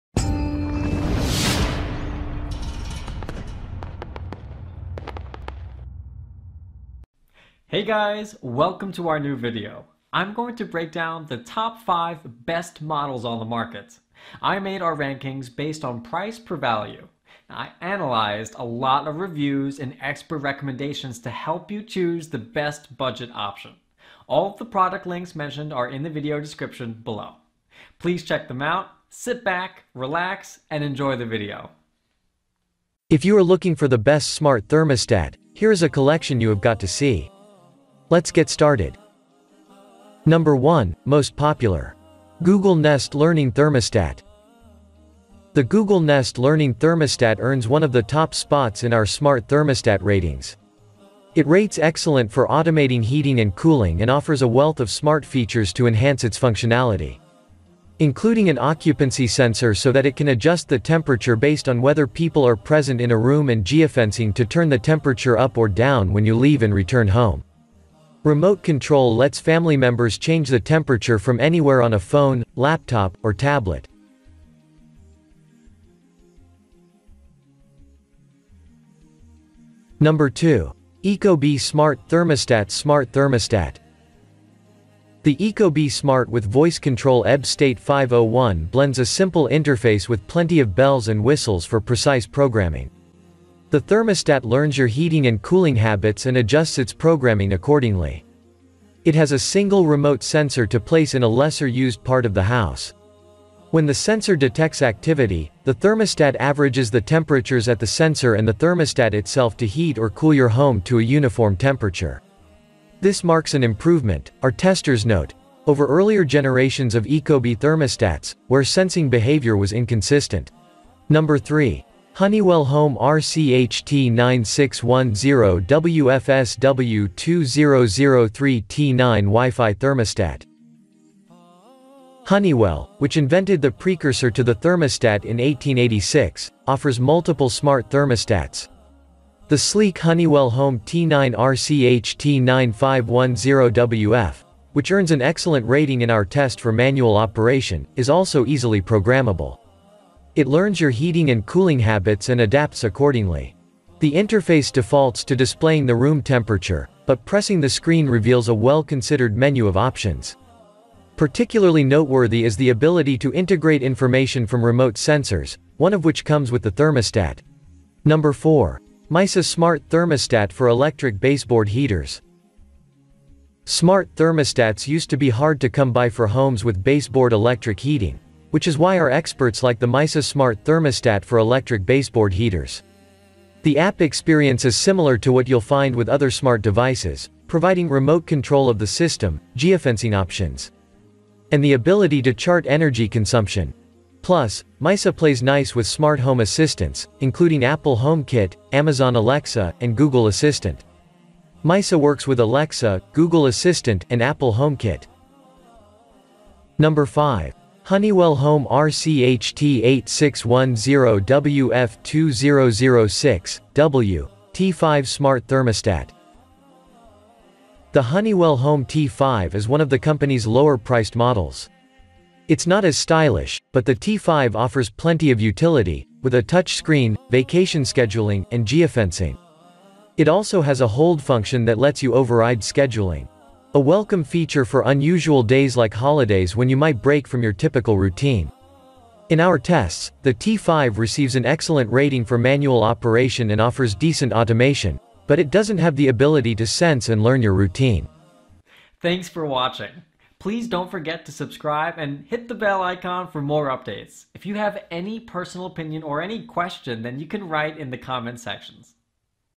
Best Smart Thermostats for 2023 [Top 5 Review]
Top 5 Best smart thermostats are here-
☑1. Google Nest Learning Thermostat
☑2. Ecobee SmartThermostat Smart Thermostat
☑3. Honeywell Home RCHT9610WFSW2003 T9 WiFi Thermostat
☑4. Mysa Smart Thermostat for Electric Baseboard Heaters
☑5. Honeywell Home RCHT8610WF2006/W, T5 Smart Thermostat

🕒Timestamps🕕
0:34 - Introduction
0:40 - Google Nest Learning Thermostat
1:19 - Ecobee SmartThermostat Smart Thermostat
2:02 - Honeywell Home RCHT9610WFSW2003 T9 WiFi Thermostat
2:48 - Mysa Smart Thermostat for Electric Baseboard Heaters
3:27 - Honeywell Home RCHT8610WF2006/W, T5 Smart Thermostat

✪ What is a smart thermostat?
Smart thermostats will be thermostats that can be utilized with home mechanization and are liable for controlling a home's warming, ventilation, and cooling. They perform comparable capacities as a Programmable thermostat as they permit the client to control the temperature of their home for the duration of the day utilizing a timetable, yet in addition contain extra highlights, like sensors and WiFi availability, that enhance the issues with programmable thermostats.